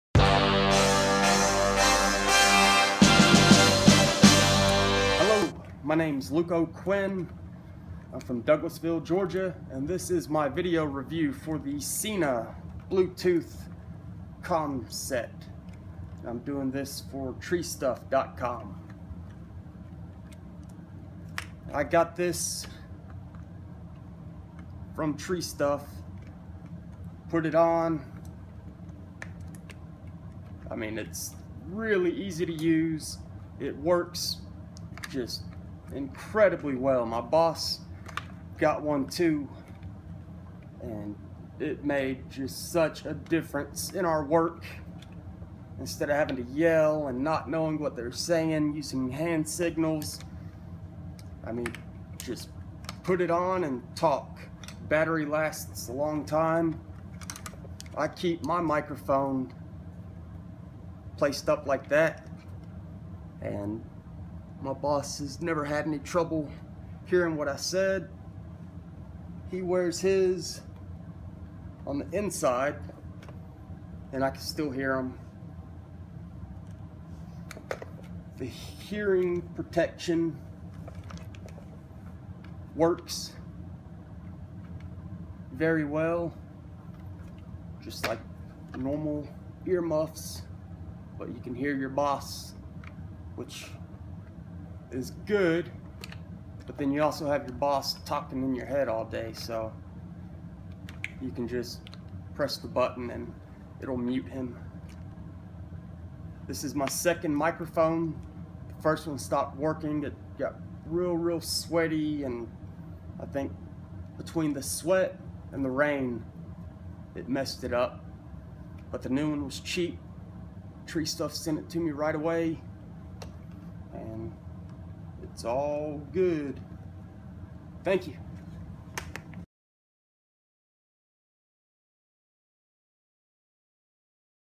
SENA Integrated Communication System: TreeStuff.com Customer Luke O'Quin's Field Review
Georgia arborist Luke O'Quin gives us his TreeStuff.com Review In The Field for the SENA Integrated Communication System.